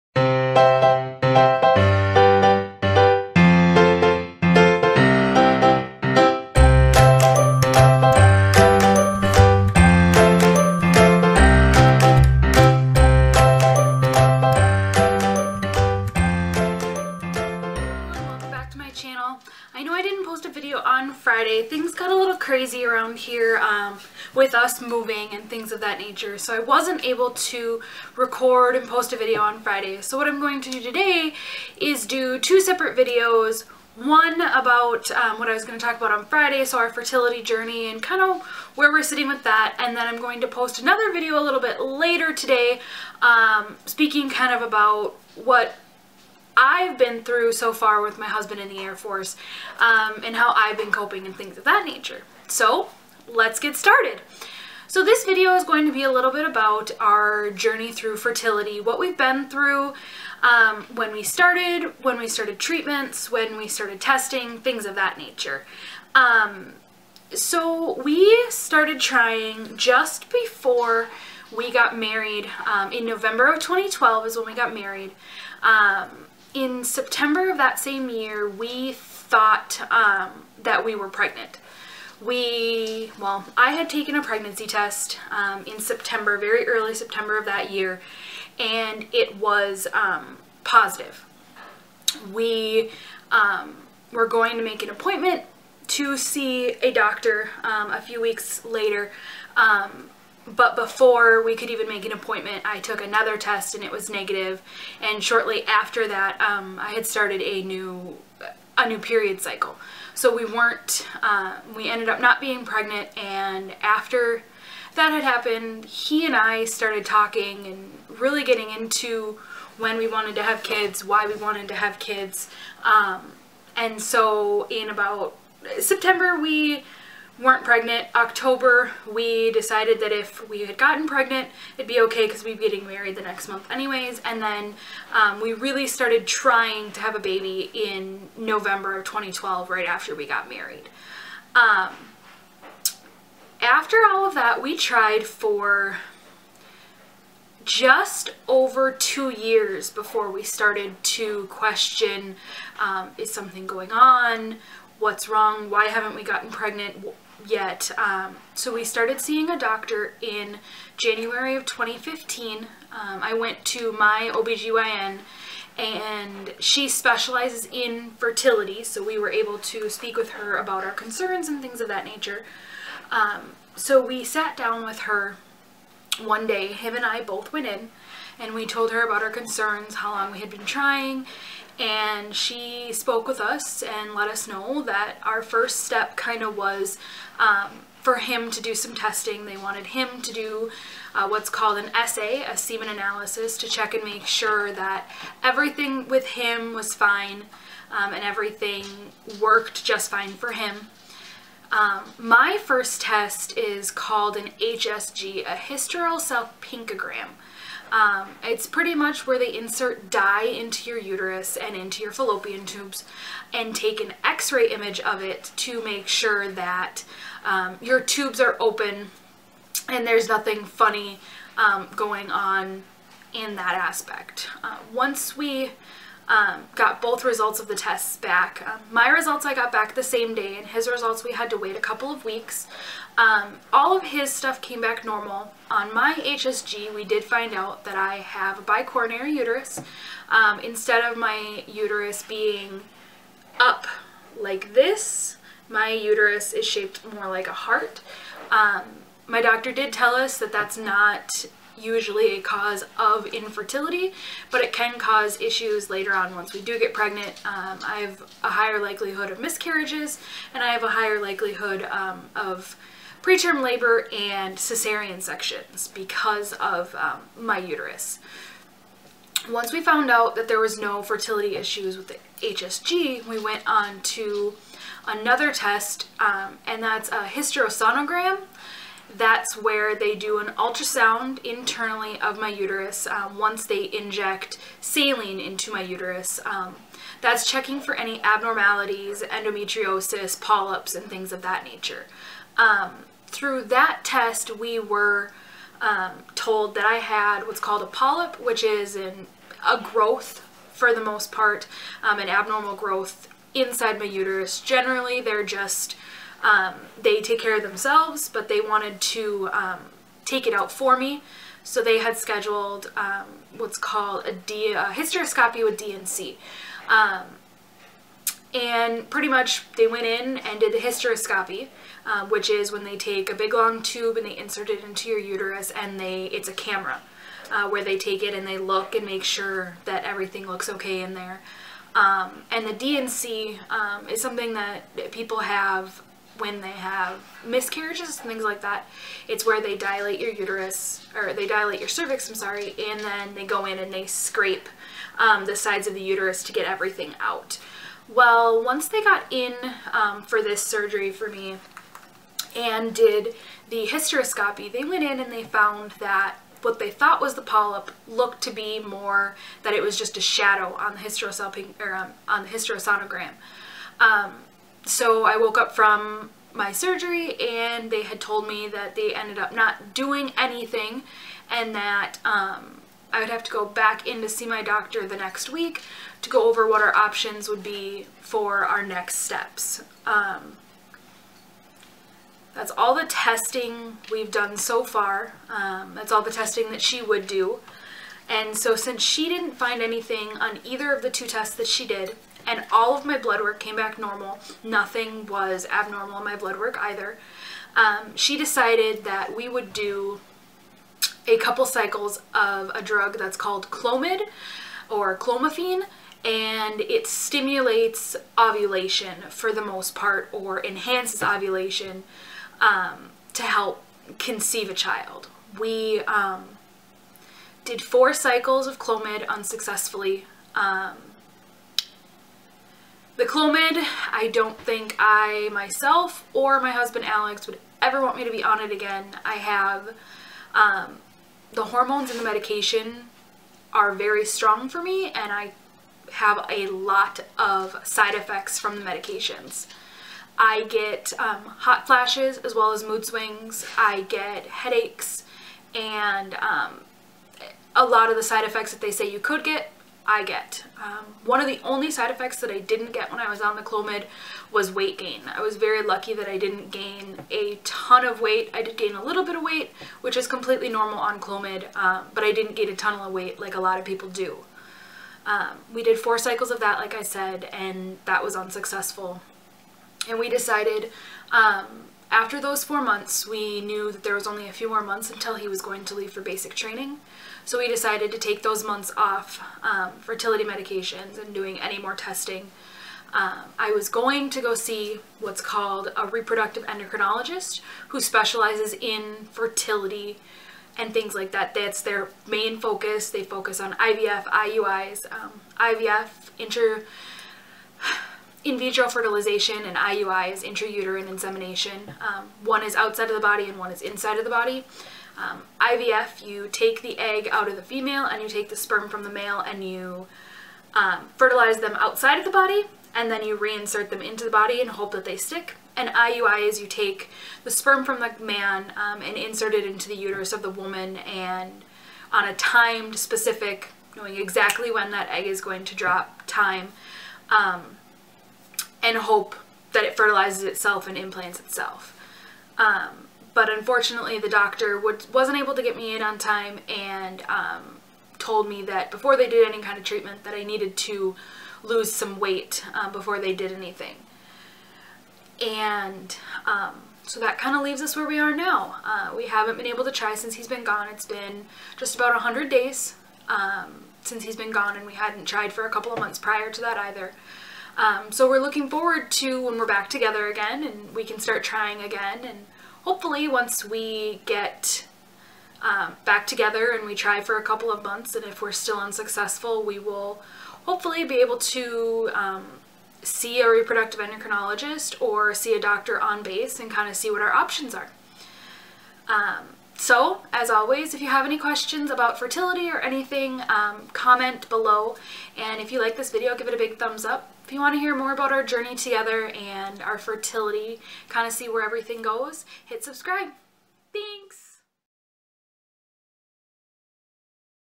Fertility Journey!
Want to know more about our fertility Journey. What testing we have done, treatment we have done and what we will do next.